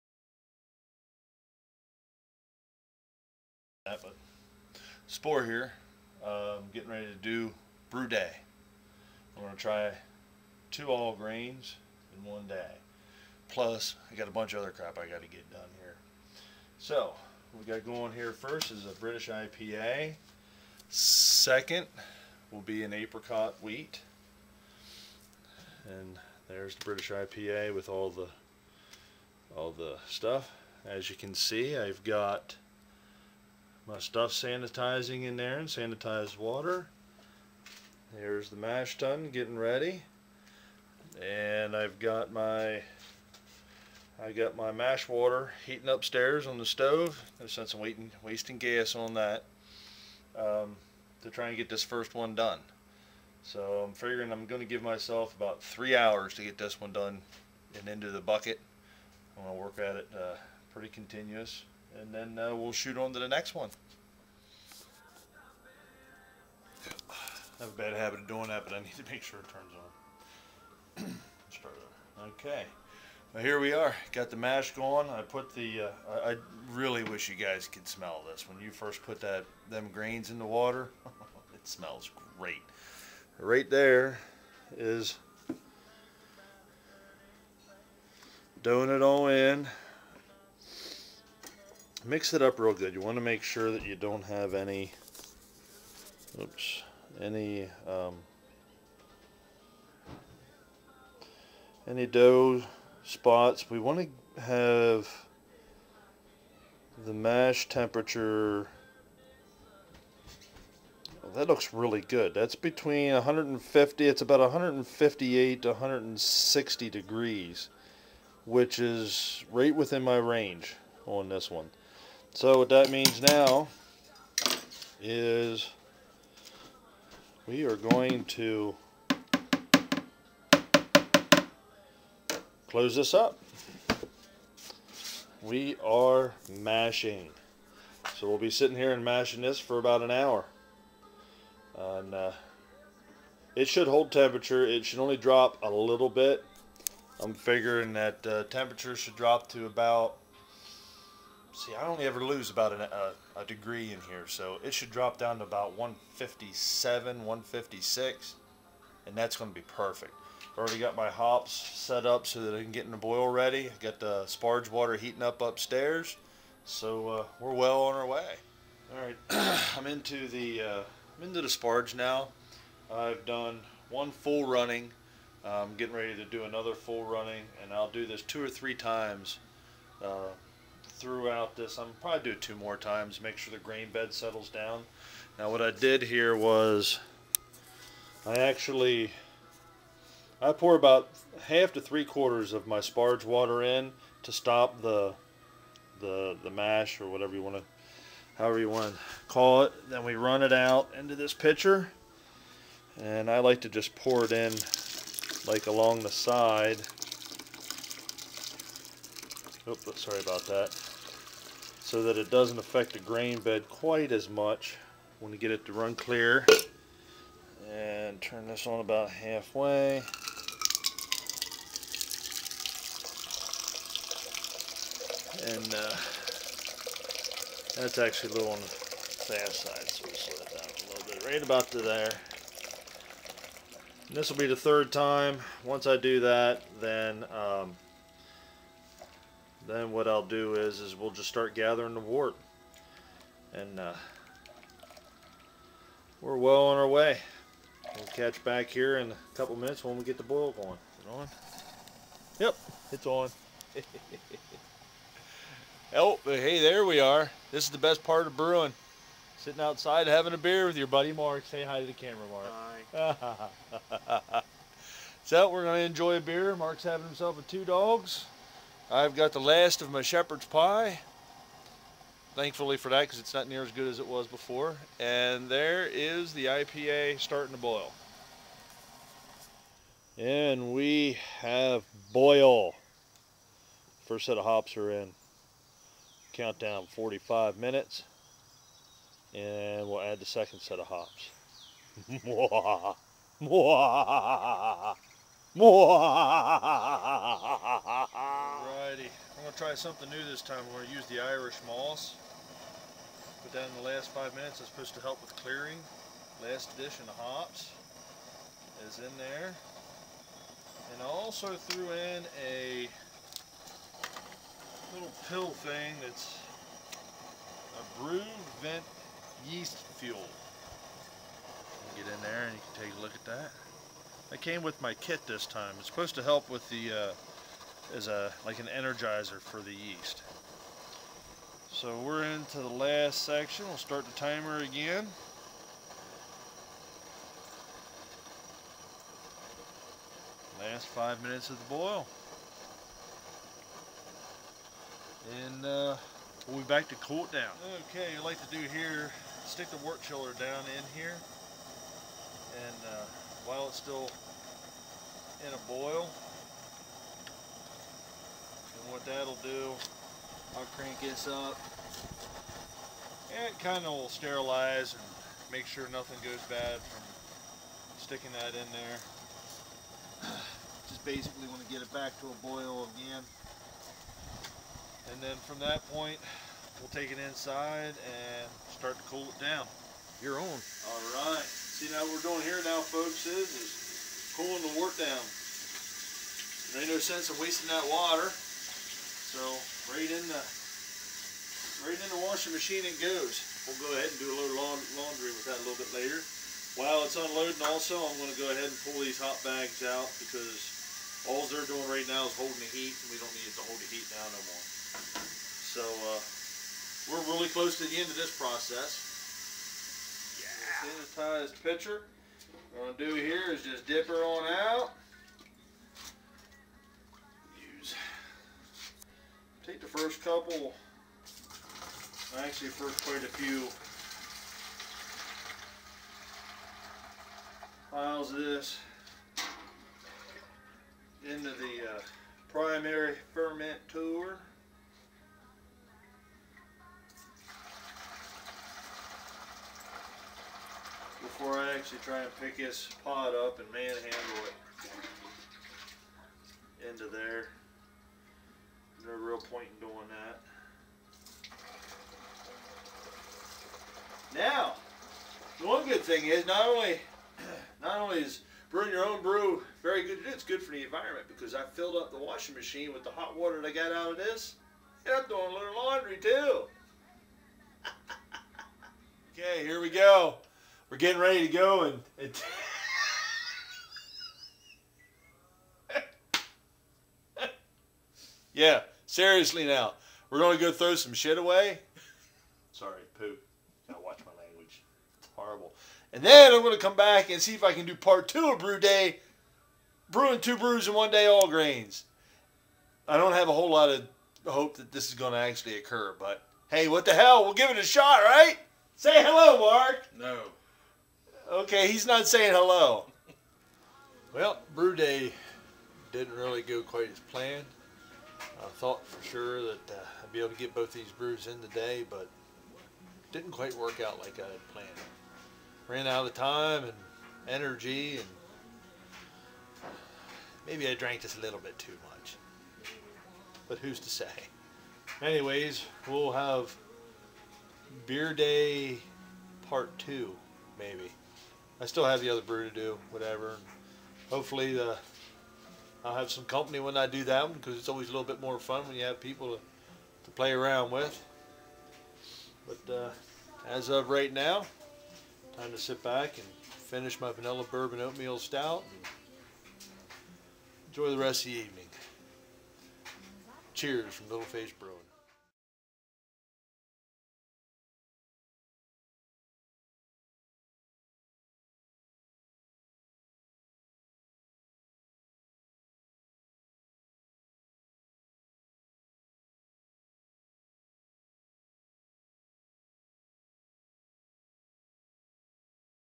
Brew Day Movie Project.wmv
This is a attempt at brewing to completely different All Grains in the same day. The first is a British IPA and the second is a Apricot Wheat. The problem is I have a boat load of other crap I need to get done at the same time. Lets see how I make out.